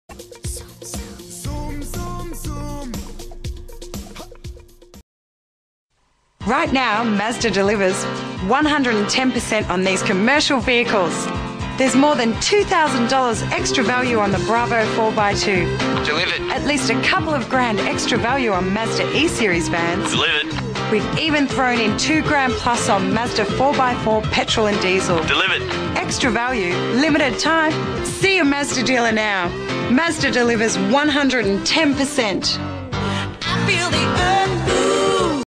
2000 Mazda E and B Series Australia CM 001
This is a Australian TV Commercial for the 2000 Mazda Bongo or E series van as it is know in Australia.